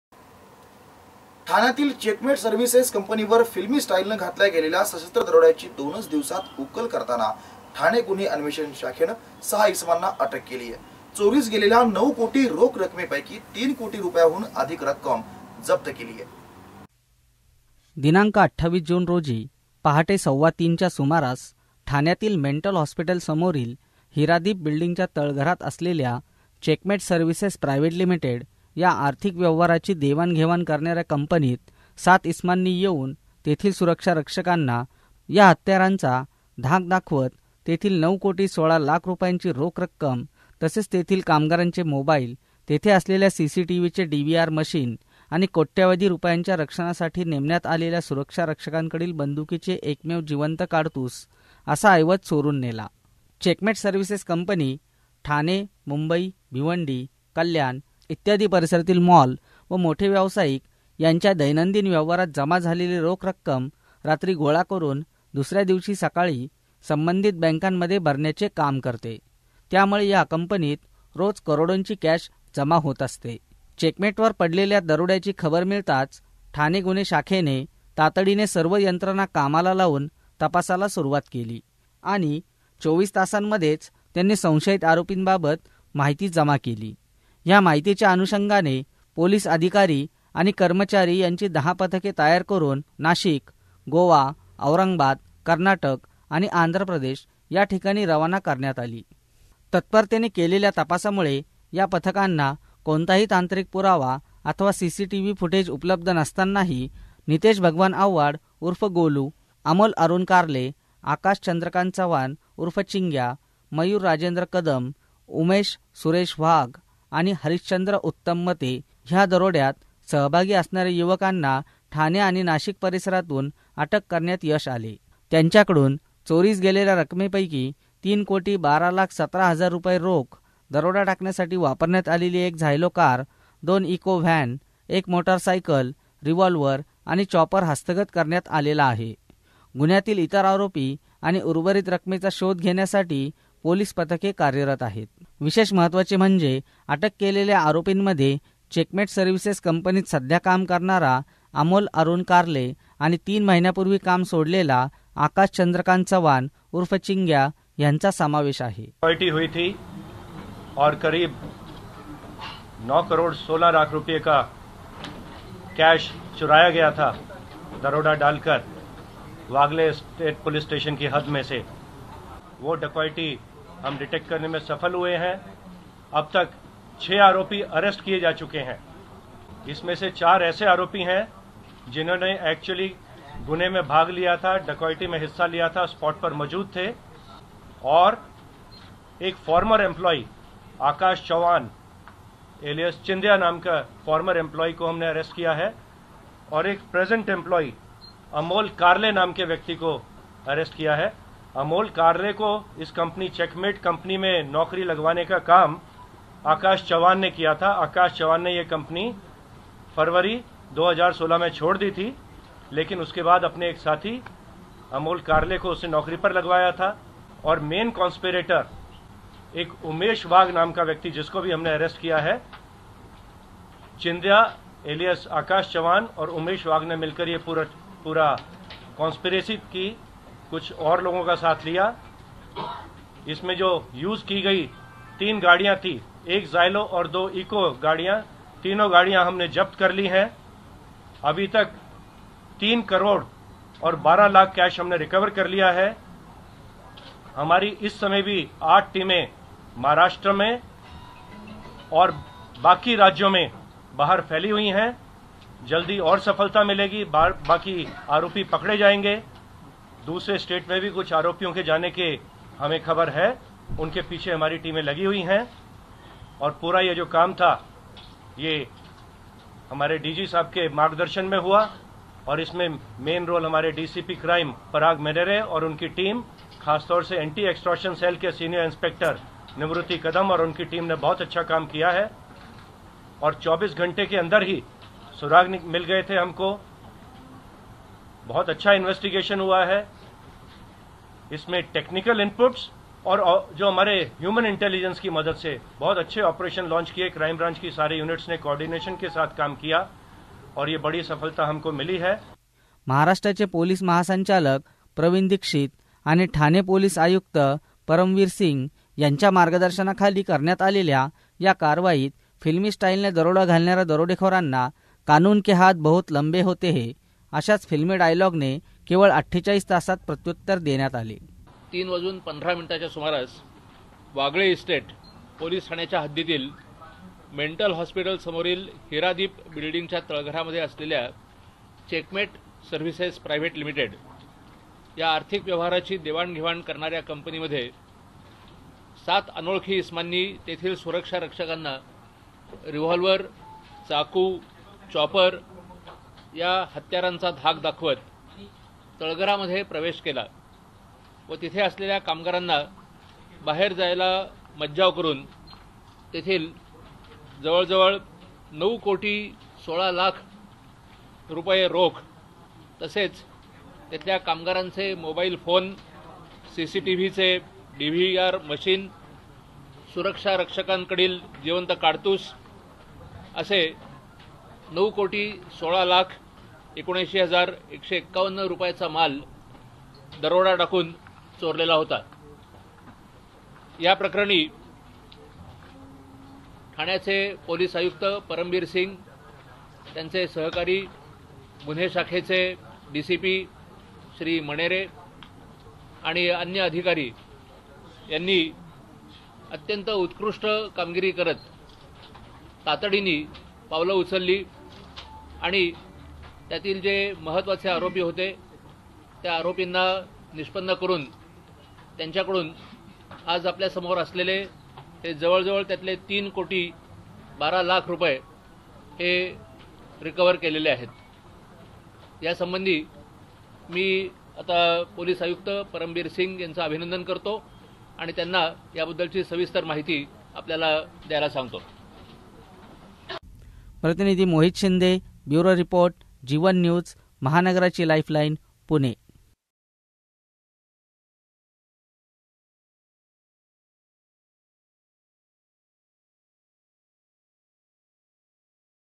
1 Thane Chekmeat Company Chori Aropi Atack
ठाण्यातील चेकमेट सर्व्हिसेस कंपनीवर फिल्मी स्टाईलने घातल्या गेलेल्या सशस्त्र दरोड्याची दोनच दिवसात उकल करतठाणे गुन्हे शाखेने सहा इसमांना अटक करून चोरीस गेलेल्या नऊ कोटी रोख रकमेपैकी तीन कोटी रुपयांहून अधिक रक्कम जप्त केली आहे